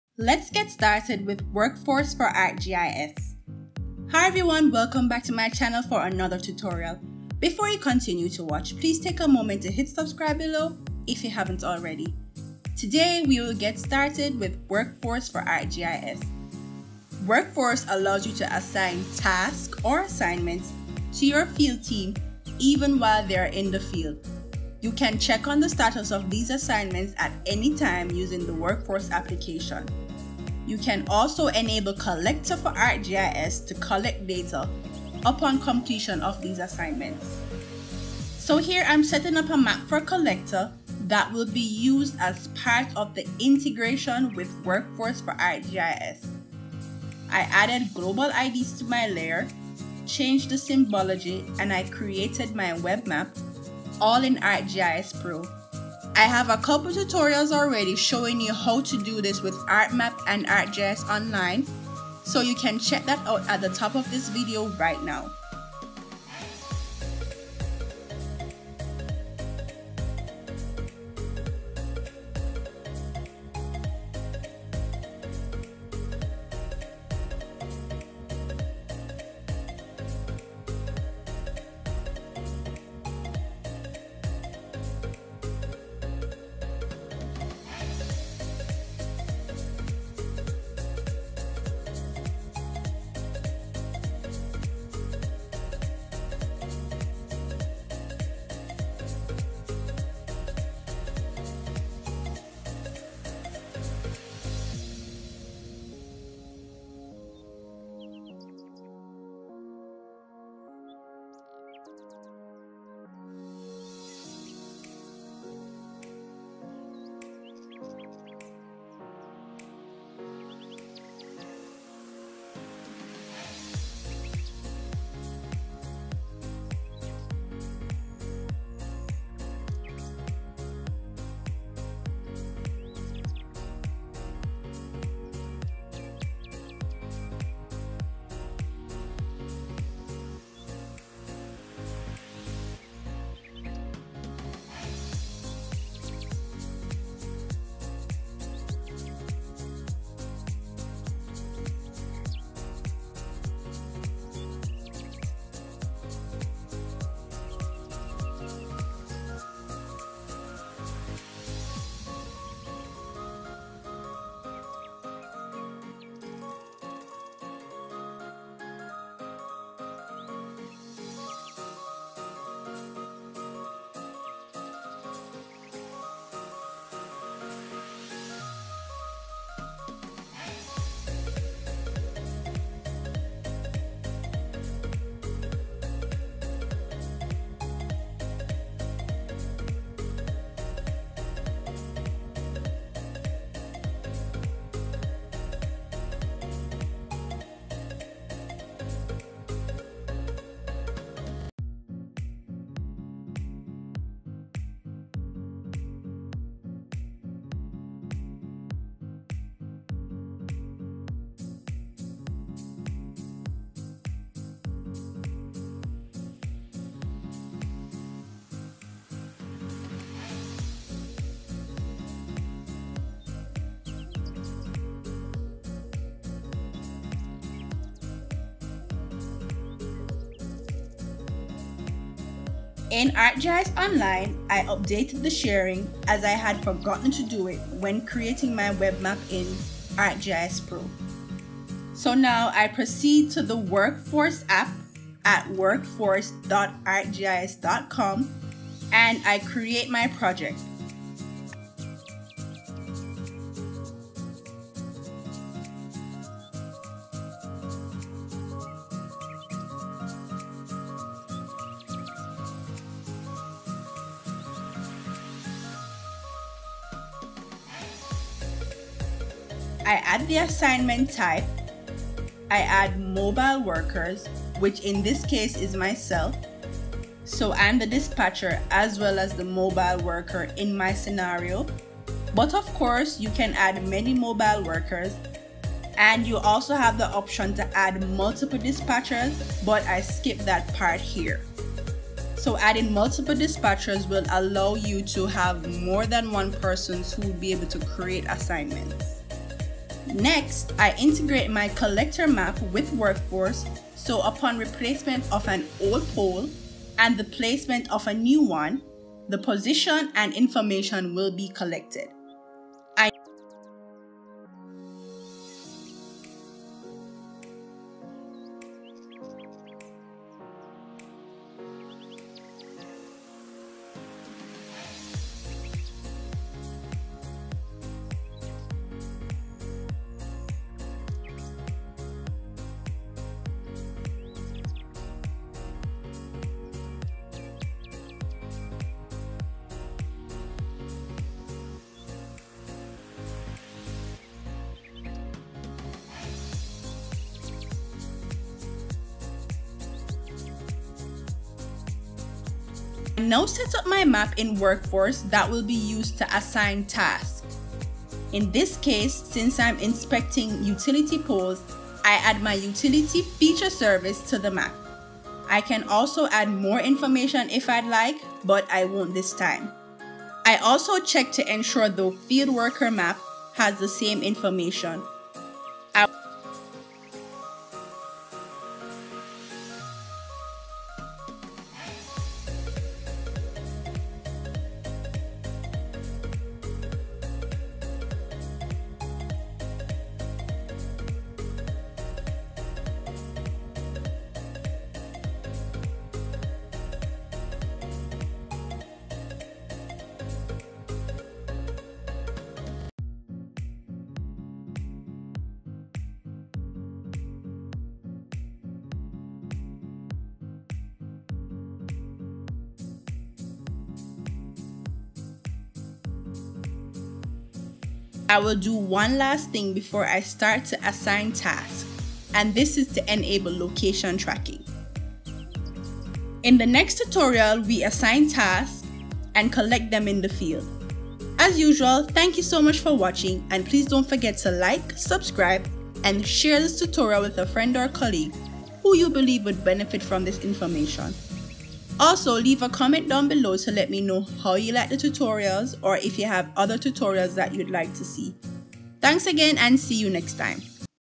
Get started with Workforce for ArcGIS
In this tutorial get started with workforce for arcgis by creating a project

Still to come on this channel: ArcGIS Pro series, Storymap series and more

Esri and the Esri Logo are licensed trademarks of Environmental Systems Research Institute, Inc.